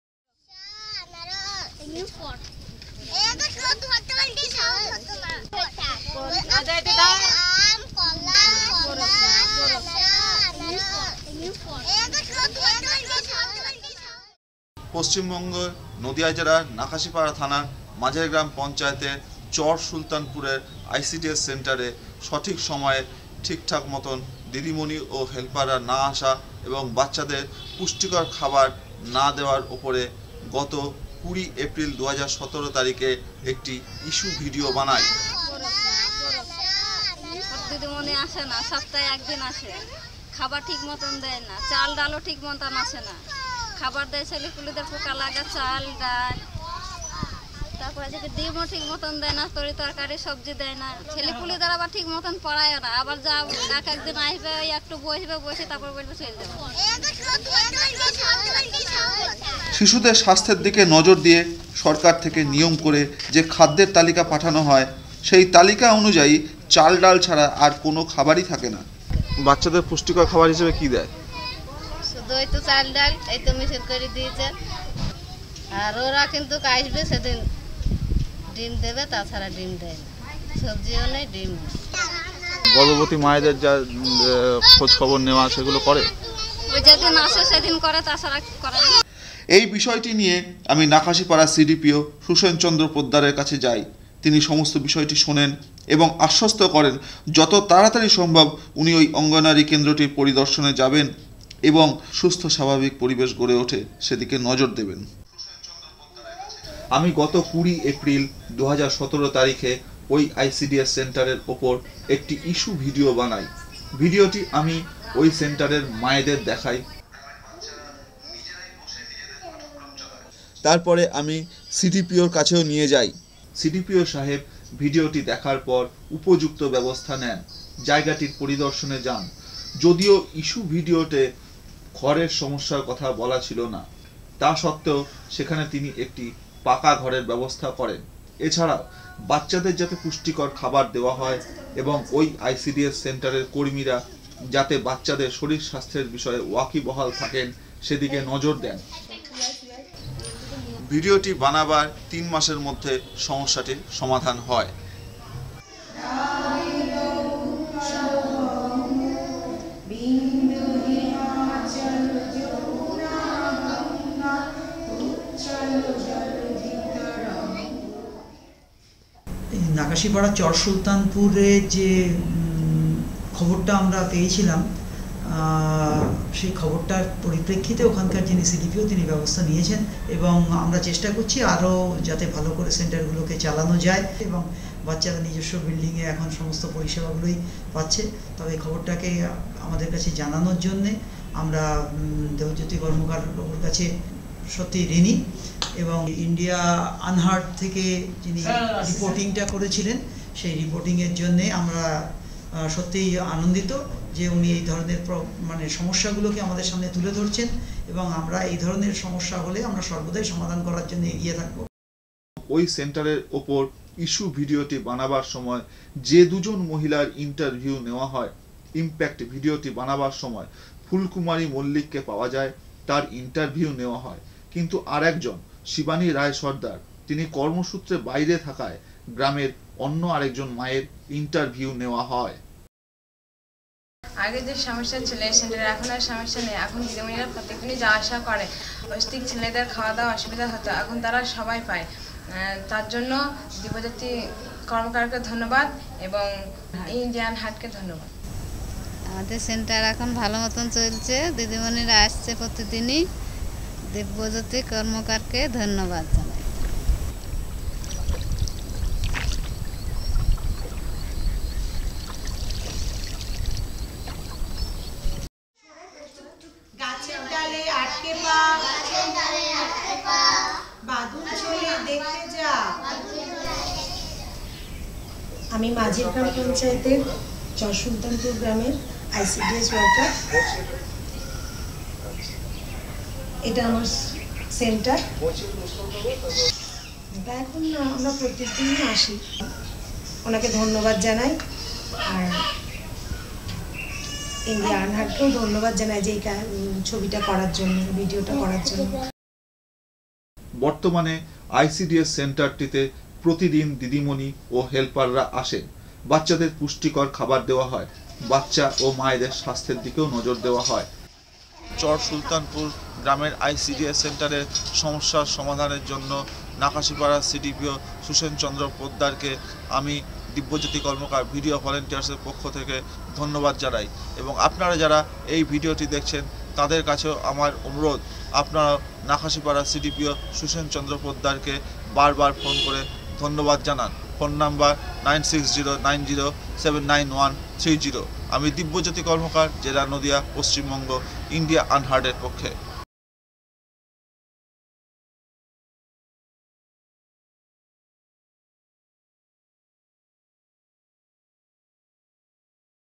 Video teaches discipline to teachers
Community Correspondent Dibyojoti Karmakar reports from West Bengal for Video Volunteers.